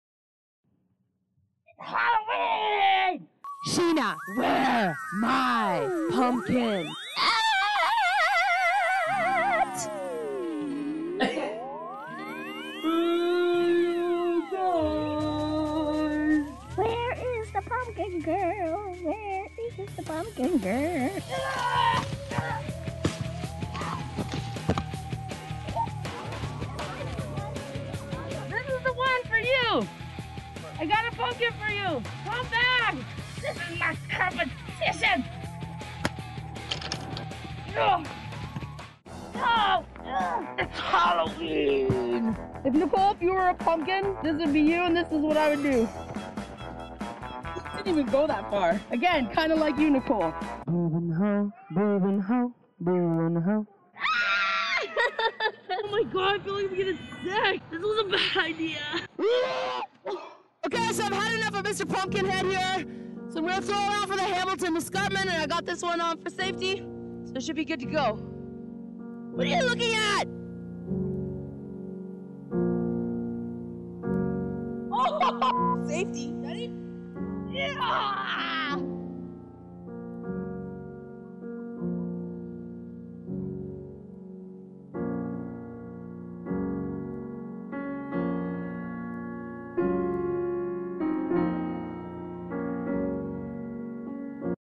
Sheena Picks Out a Pumpkin
TheDarynJones. Sheena Snively visits a pumpkin patch to pick out a pumpkin for Halloween. Daryn Jonens